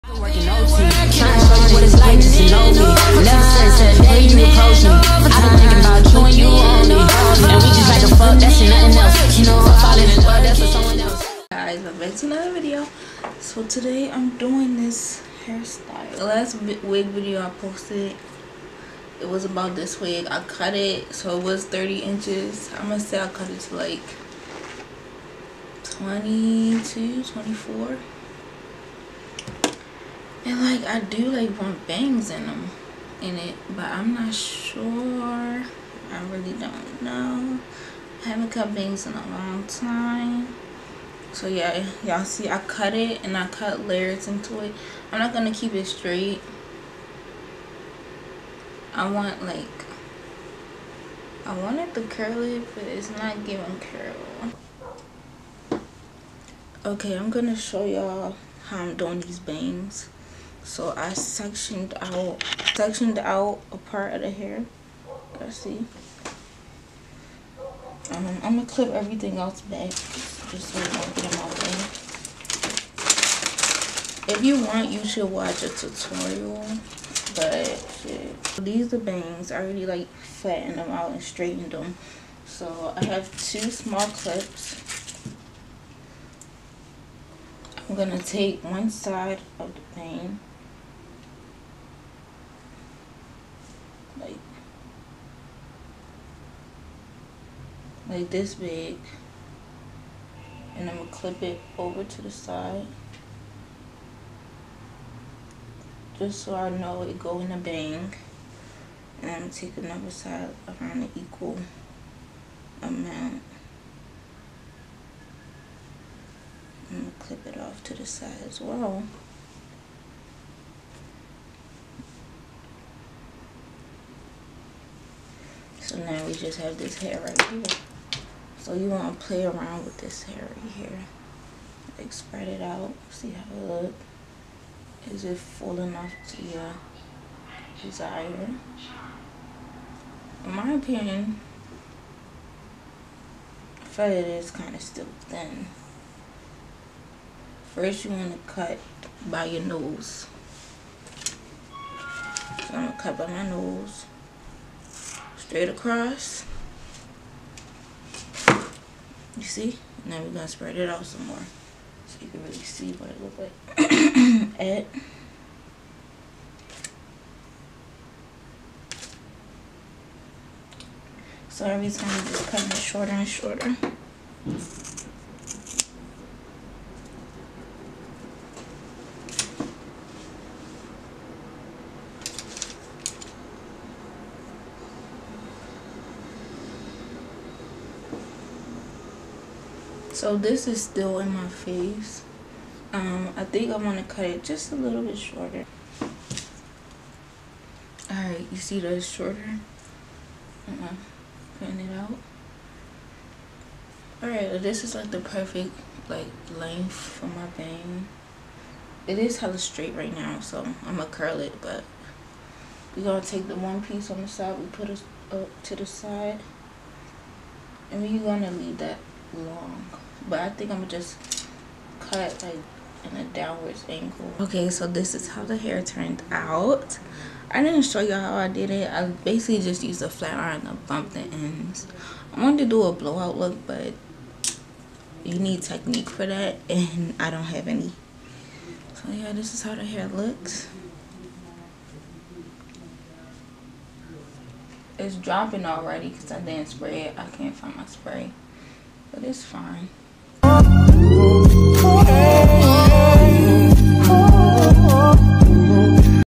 How I Cut Bangs into my wig ✂️
Hey guys so today I did a short video just showing y'all how I cut bangs into a wig. I can do a more detailed video but that was on the fly fr fr.